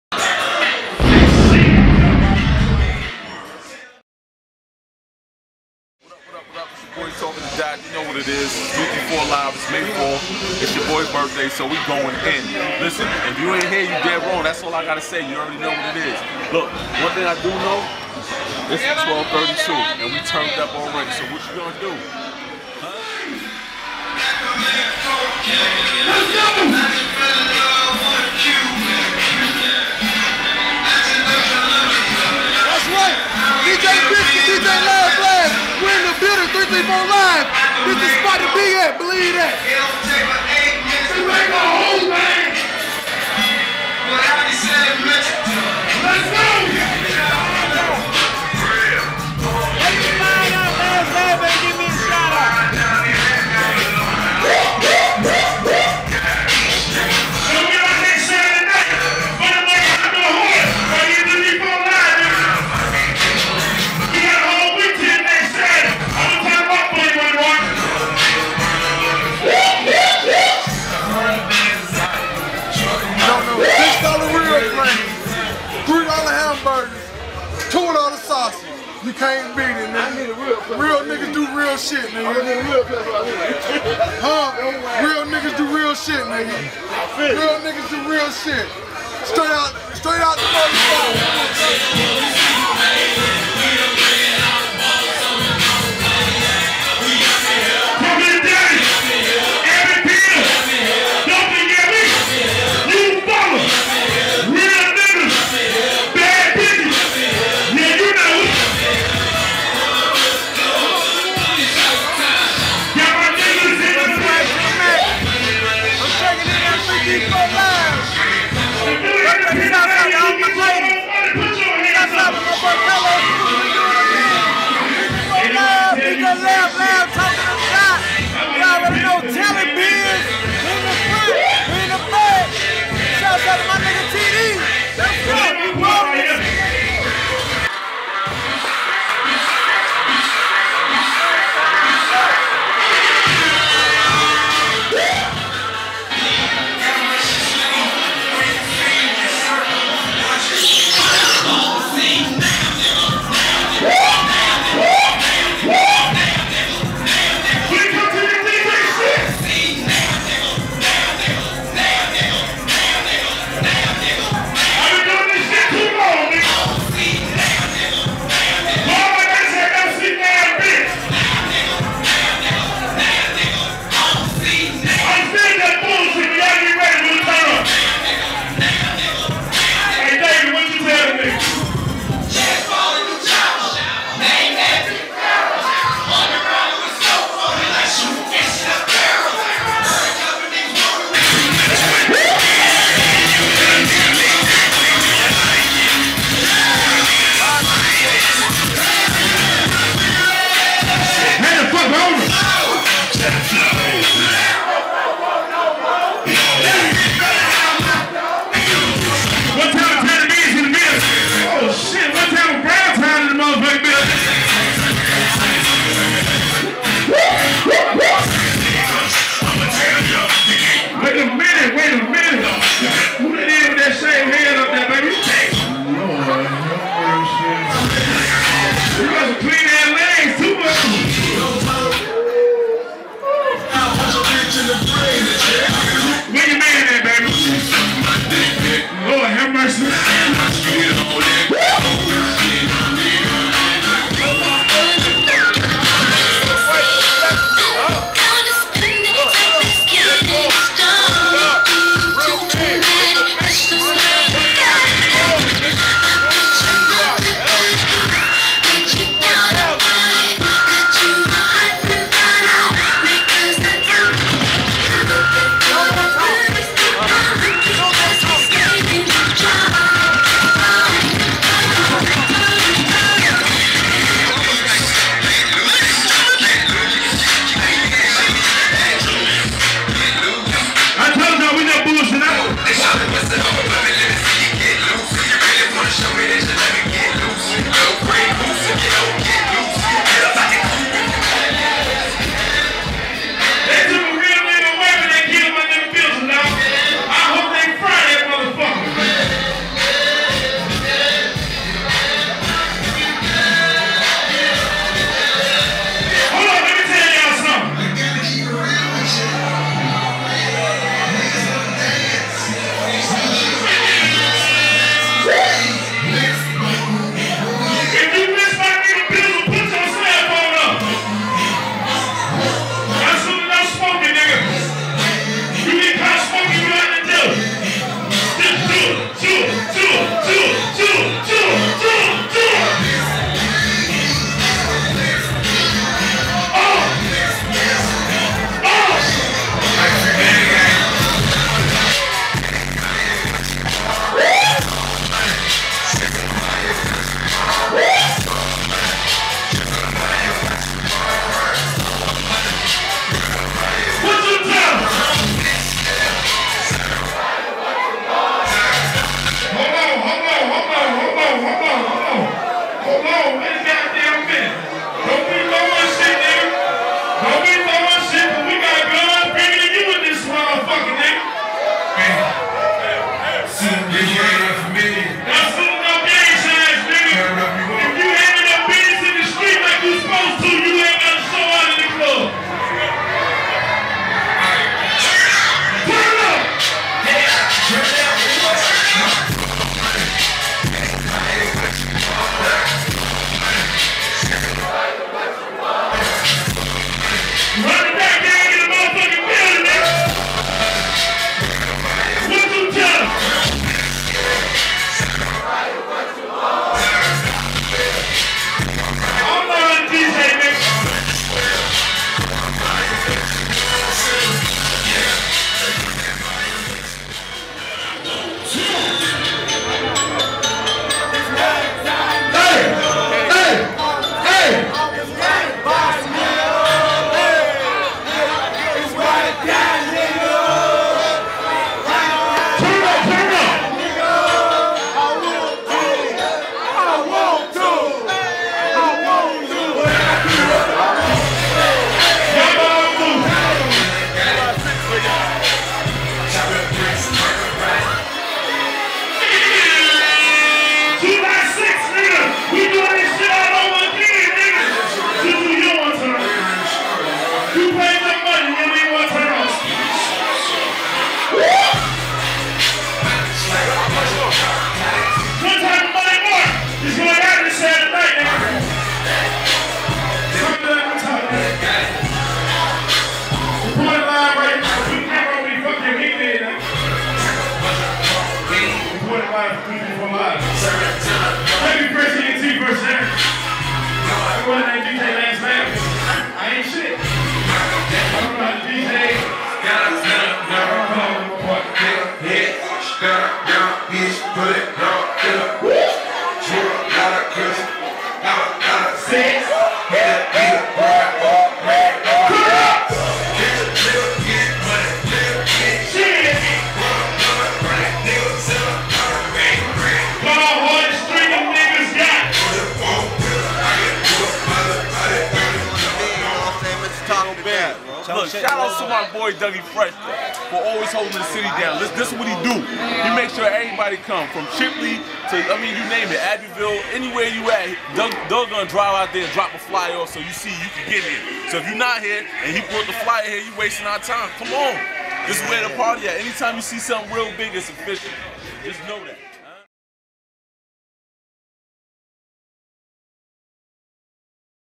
REAL BIG MAY 4TH/ TALK OF THE DOT BIRTHDAY BASH
Talk Of The Dot Birthday Bash W/ Douggie Fresh & DJ LastLaff
- Real Big May 4th @ 334 Live Dothan AL